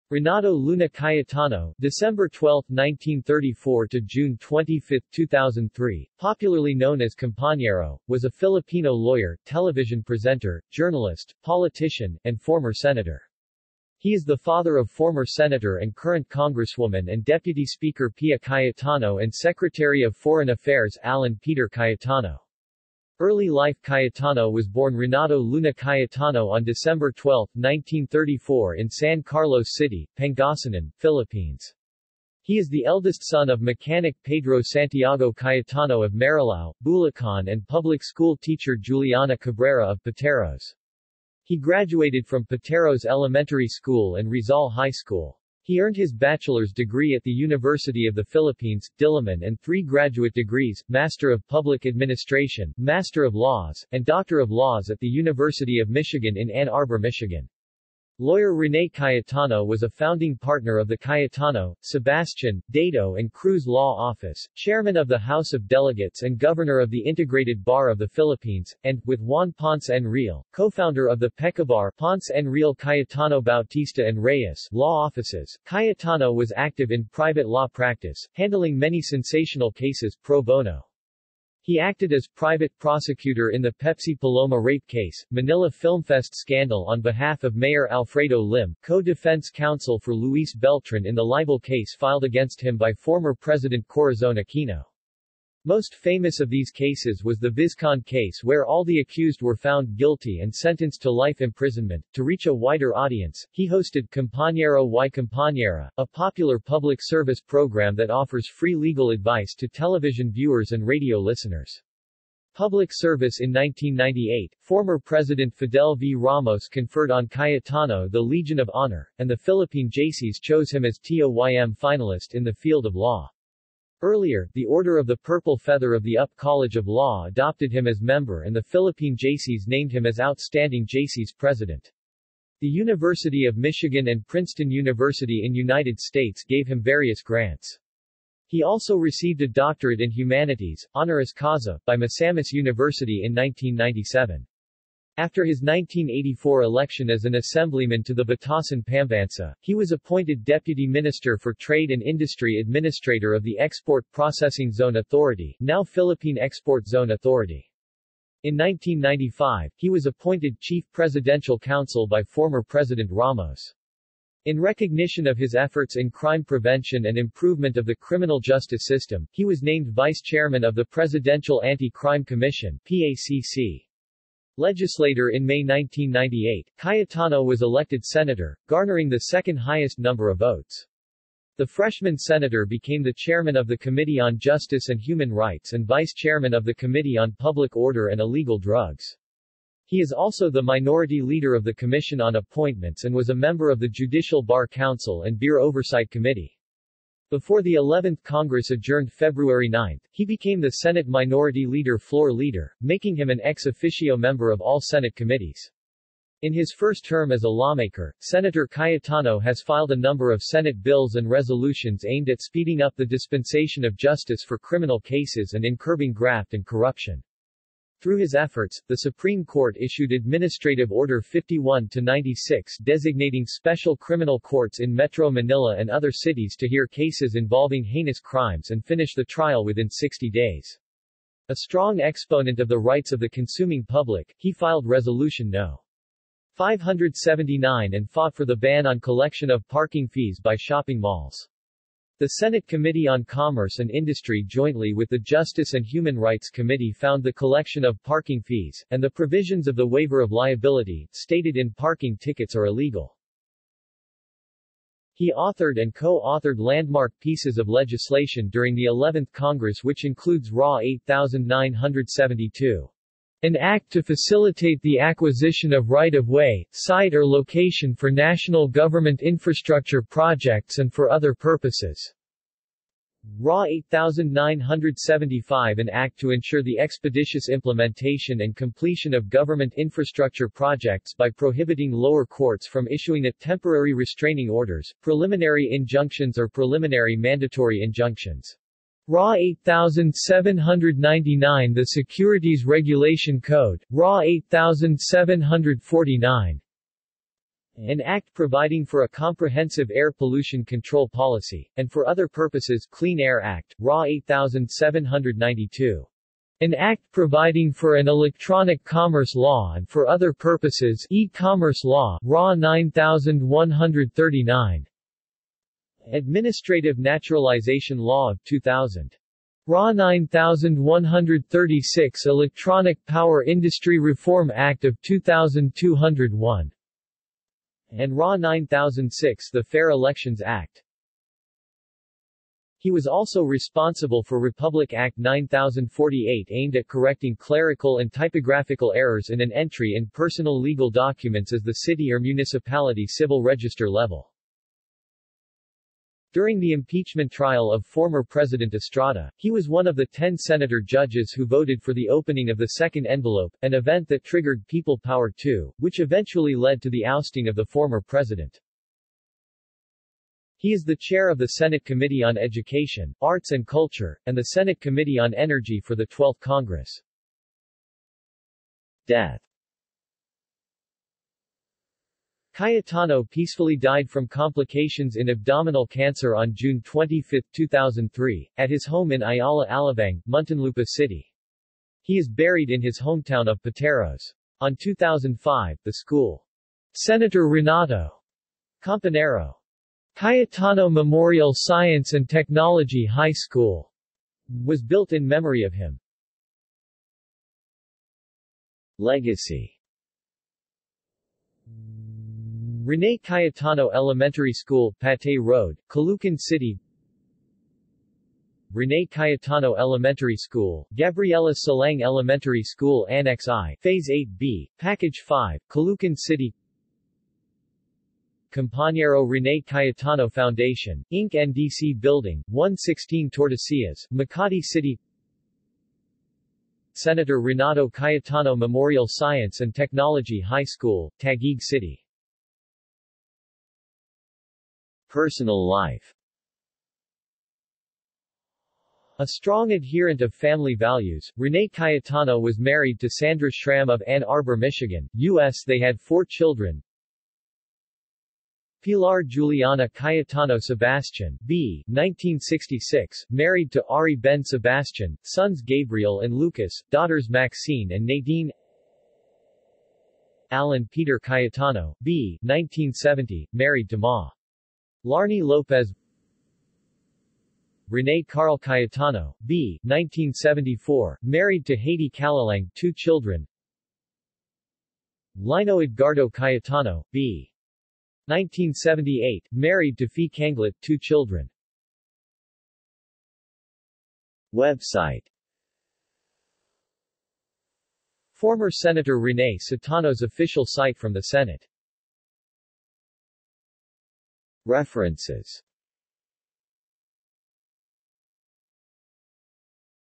Rene Cayetano | Wikipedia audio article
This is an audio version of the Wikipedia Article:
Rene Cayetano

SUMMARY
Renato Luna Cayetano (December 12, 1934 – June 25, 2003), popularly known as Compañero, was a Filipino lawyer, television presenter, journalist, politician, and former Senator. He is the father of former Senator and current congresswoman and Deputy Speaker Pia Cayetano and Secretary of Foreign Affairs Alan Peter Cayetano.